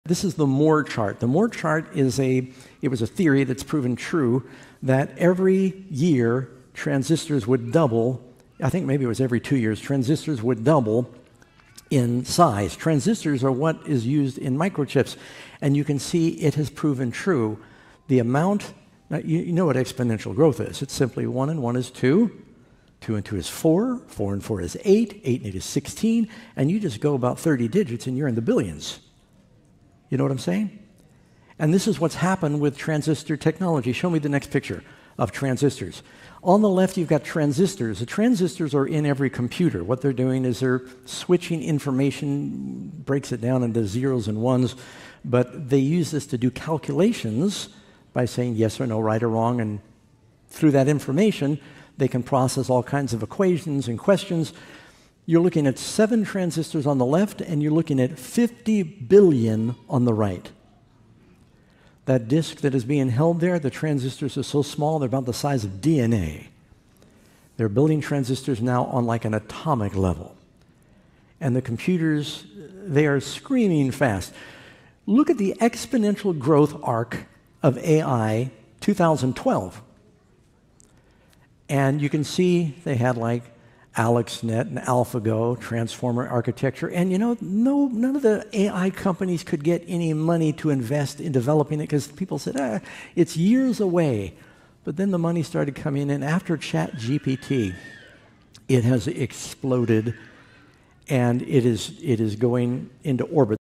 The Exponential Growth of AI
Witness the mind-blowing speed at which AI is evolving! From doubling transistors to AI assistants, the future is now!